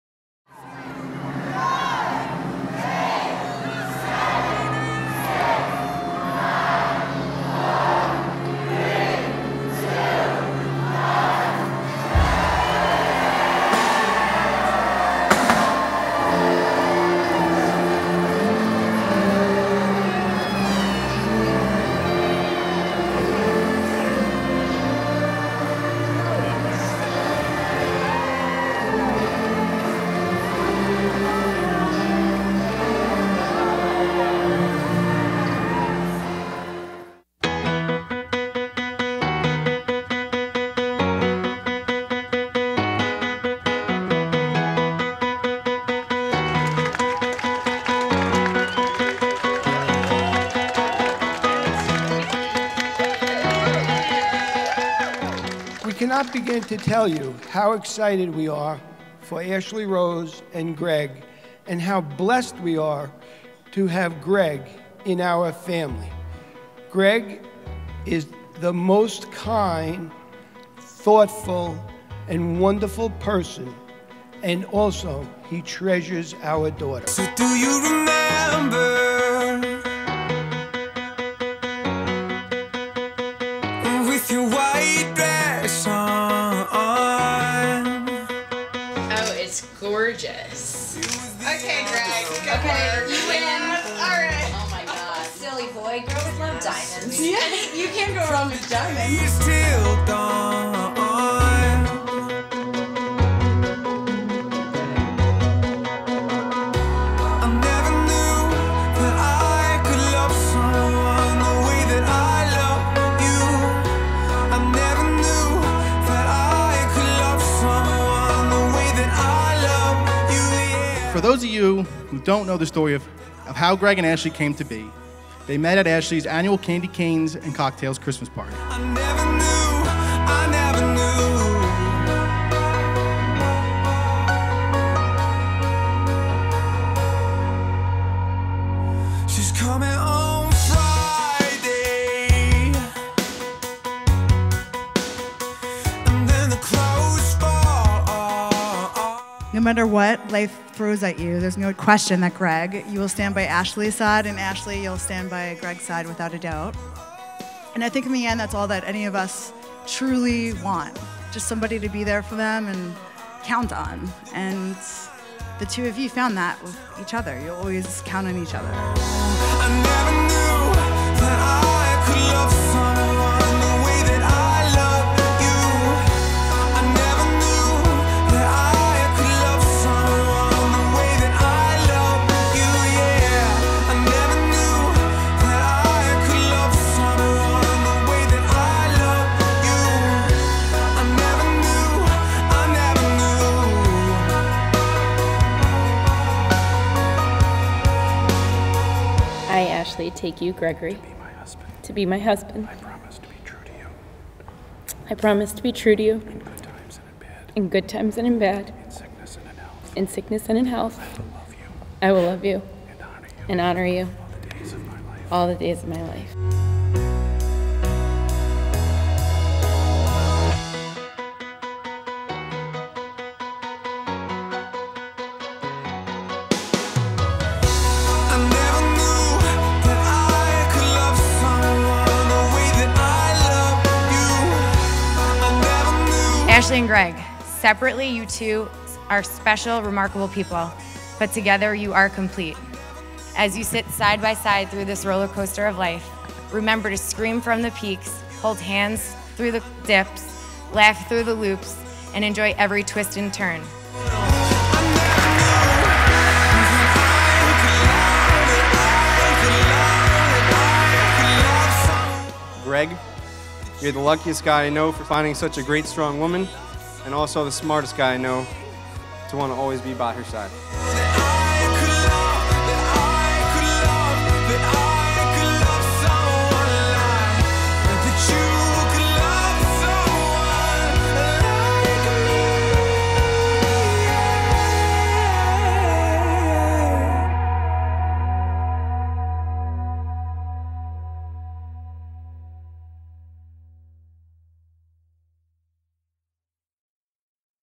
Please Touch Museum + New Year's Eve Wedding {Ashley + Gregory Highlight Film}
Blossom Productions presents a brief look at Ashely and Gregory's New Year's Eve wedding at Crowne Plaza, St. Paul's Parish, and the Please Touch Museum.